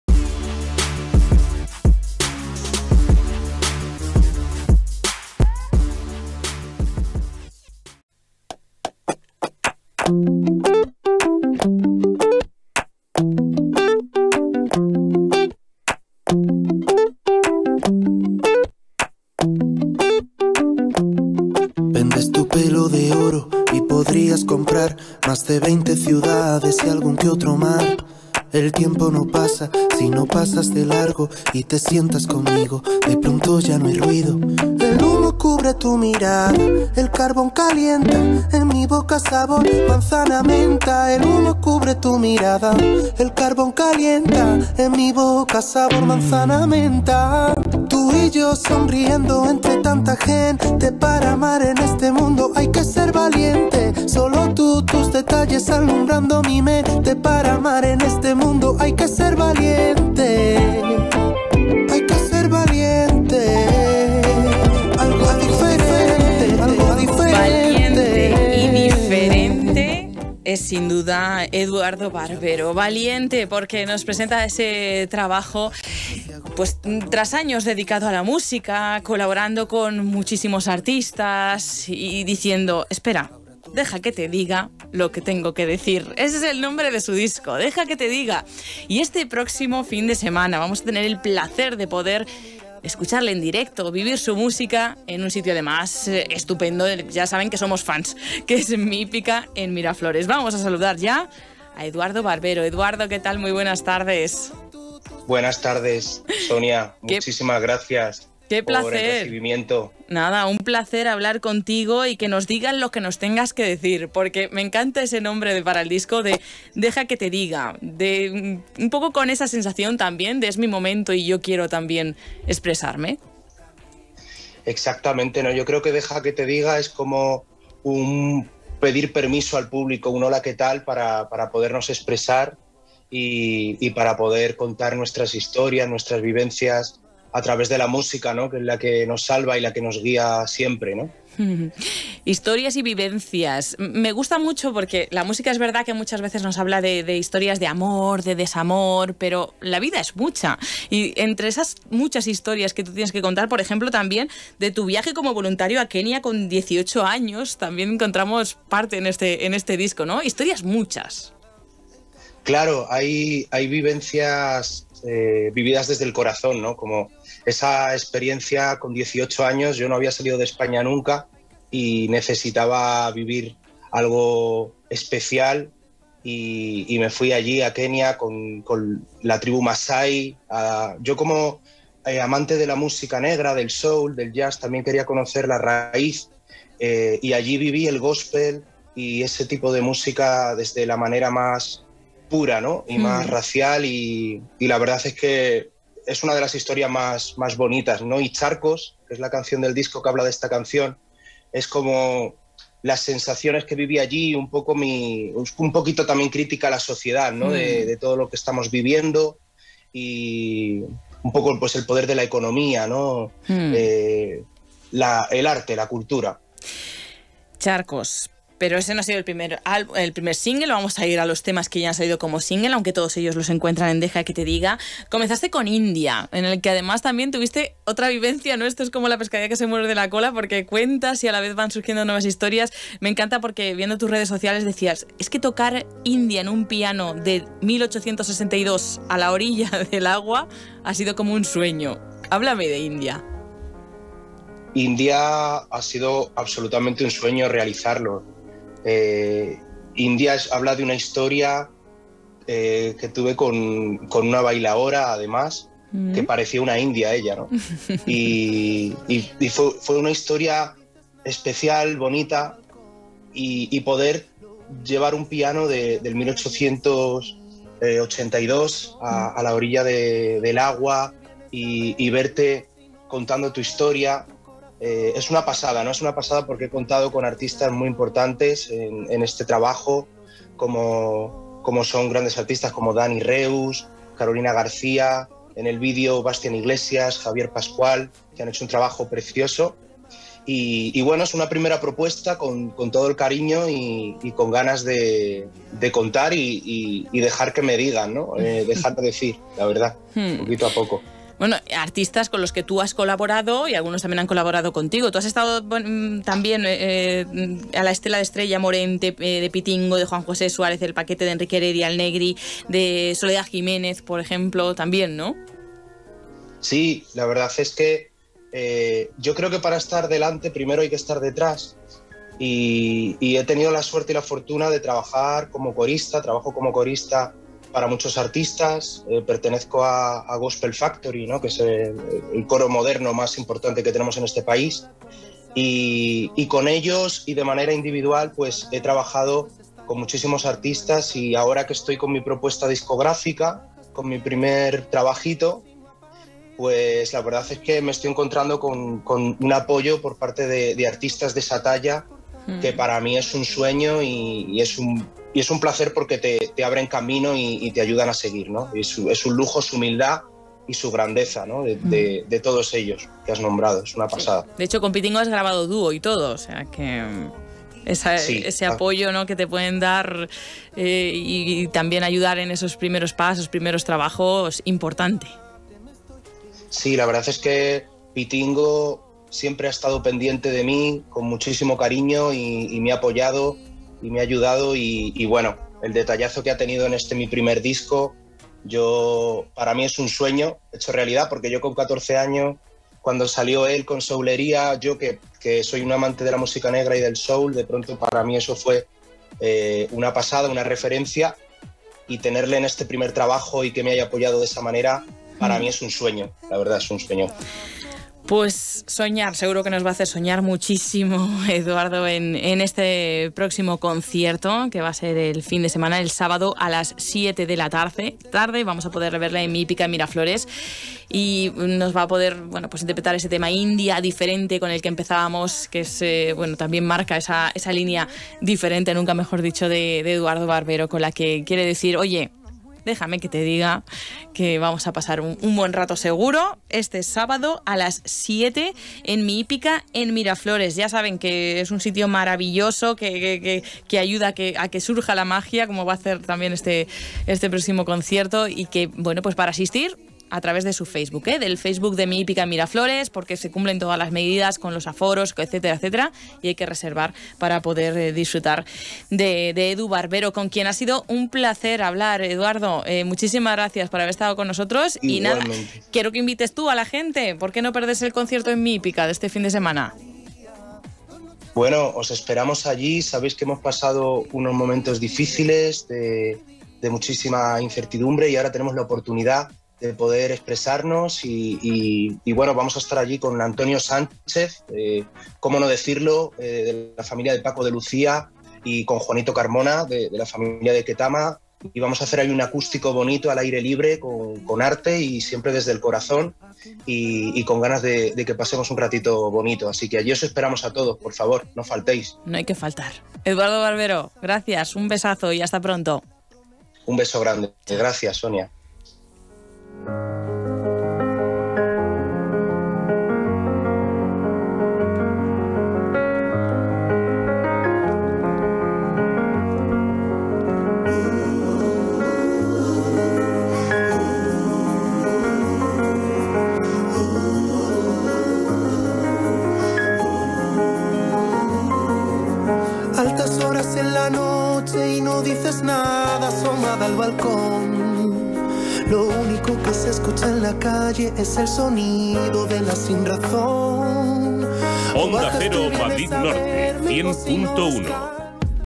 Mi Hípica en Miraflores Concierto Eduardo Barbero
Charlamos con Edu Barbero, la apuesta musical de este fin de semana en Mi Hípica en Miraflores, con un sonido de lo más personal, mezclando flamenco y música negra.